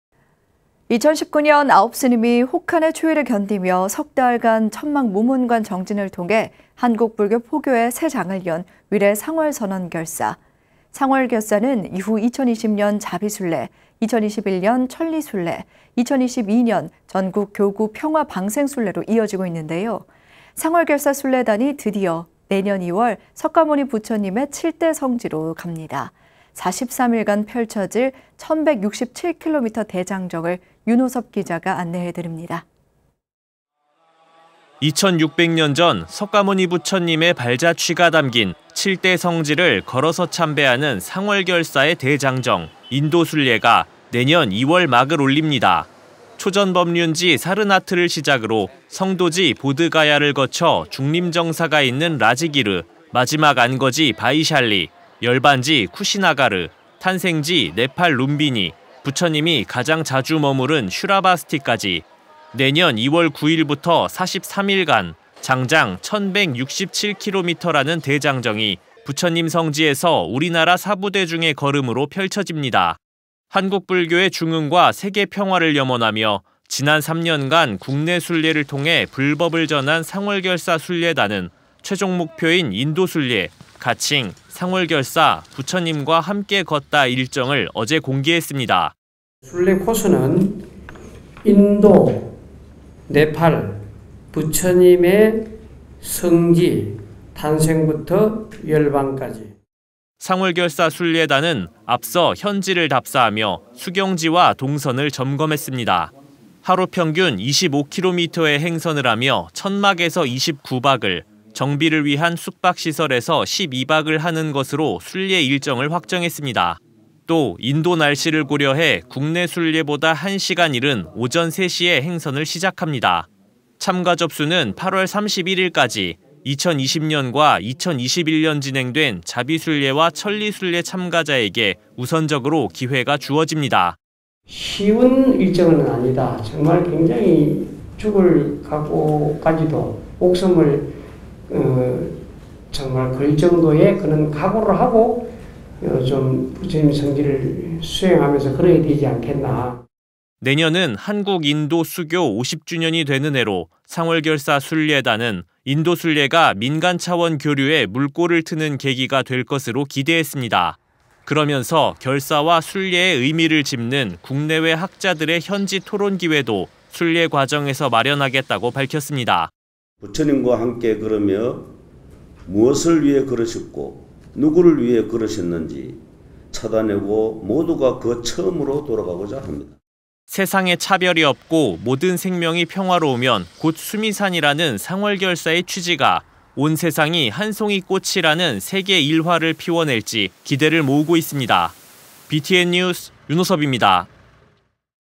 [BTN뉴스] 상월결사 ‘43일간 1167km’ 인도순례 확정
〔앵커〕

2019년 아홉 스님이 혹한의 추위를 견디며 석 달간 천막 무문관 정진을 통해 한국불교 포교에 새장을 연 위례 상월선원결사. 상원결사는 이후 2020년 자비순례, 2021년 천리순례, 2022년 전국 교구 평화 방생순례로 이어지고 있는데요, 상월결사 순례단이 드디어 내년 2월 석가모니 부처님의 7대 성지로 갑니다. 43일간 펼쳐질 1,167km 대장정을 윤호섭 기자가 안내합니다.
〔리포트〕

2600년 전 석가모니 부처님의 발자취가 담긴 7대 성지를 걸어서 참배하는 상월결사의 대장정 인도순례가 내년 2월 막을 올립니다.

초전법륜지 사르나트를 시작으로 성도지 보드가야를 거쳐 죽림정사가 있는 라지기르, 마지막 안거지 바이샬리, 열반지 쿠시나가르, 탄생지 네팔 룸비니, 부처님이 가장 자주 머무른 슈라바스티까지.

내년 2월 9일부터 43일간 장장 1,167km라는 대장정이 부처님 성지에서 우리나라 사부대중의 걸음으로 펼쳐집니다.

한국불교의 중흥과 세계평화를 염원하며 지난 3년간 국내 순례를 통해 불법을 전한 상월결사 순례단은 최종 목표인 인도순례 가칭 ‘상월결사, 부처님과 함께 걷다’ 일정을 어제 공개했습니다.

호산스님 / 상월결사 총도감
(순례코스는 인도, 네팔 부처님의 성지 탄생부터 열반까지입니다.)

상월결사 순례단은 앞서 현지를 답사하며 숙영지와 동선을 점검했습니다.

하루 평균 25km의 행선을 하며 천막에서 29박을, 정비를 위한 숙박시설에서 12박을 하는 것으로 순례일정을 확정했습니다.

또 인도 날씨를 고려해 국내 순례보다 1시간 이른 오전 3시에 행선을 시작합니다.

참가접수는 8월 31일까지, 2020년과 2021년 진행된 자비순례와 천리순례 참가자에게 우선적으로 기회가 주어집니다.

(쉬운 일정은 아닙니다. 정말 굉장히 죽을 각오까지도, 목숨을 걸 정도의 각오를 하고 부처님 성지를 수행하면서 걸어야 하지 않겠나...)

내년은 한국-인도 수교 50주년이 되는 해로, 상월결사 순례단은 인도순례가 민간차원 교류에 물꼬를 트는 계기가 될 것으로 기대했습니다.

그러면서 결사와 순례의 의미를 짚는 국내외 학자들의 현지 토론 기회도 순례 과정에서 마련하겠다고 밝혔습니다.

설암스님 / 삼보사찰 천리순례 4조 조장(인도순례 취지문 낭독)
(부처님과 함께 걸으며 무엇을 위해 걸으셨고 누구를 위해 걸으셨는지 찾아내고 모두가 그 처음으로 돌아가고자 합니다.)

“세상에 차별이 없고 모든 생명이 평화로우면 곧 수미산”이라는 상월결사의 취지가 ‘온 세상이 한 송이 꽃’이라는 세계일화를 피워낼지 기대를 모으고 있습니다.

BTN 뉴스 윤호섭입니다.

#1,167km 대장정

●BTN 유튜브를 후원해주세요.